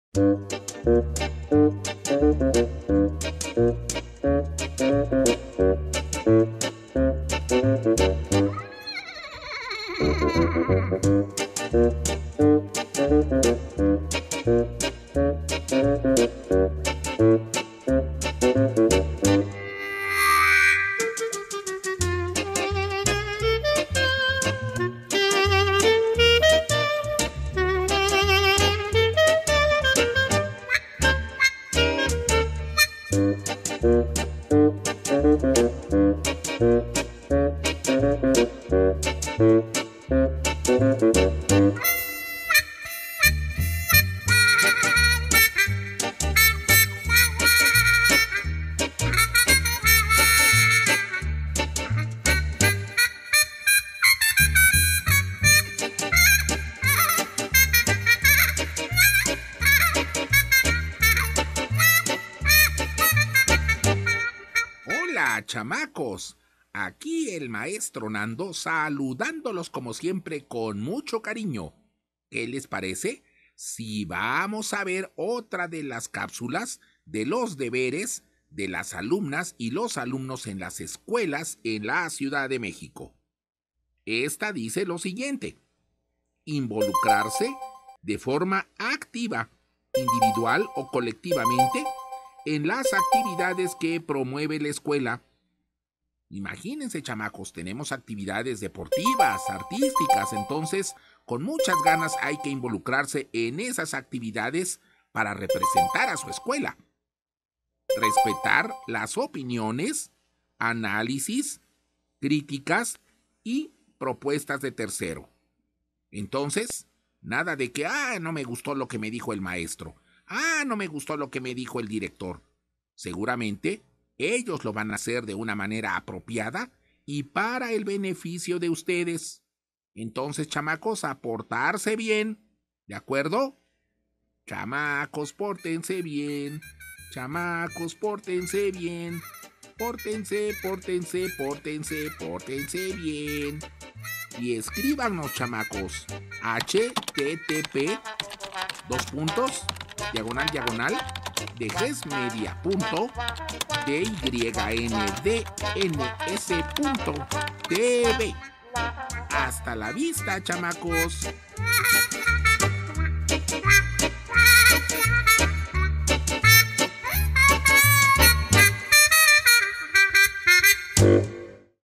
Marco para la Convivencia Escolar/Deberes - Cápsula 9/20190130-CE1819
Marco para la Convivencia Escolar. Deberes - Cápsula 9 - Deberes. Invitado: Maestro Nando. Conduce: Prof. Fernando Pérez. Fecha de emisión: 30 de Enero del 2018.Este y otros programas los puedes ver Producciones con fines educativos a cargo del Lic. Ernesto Gutiérrez Garcés - Director General de Educación Secundaria Técnica en la CDMX.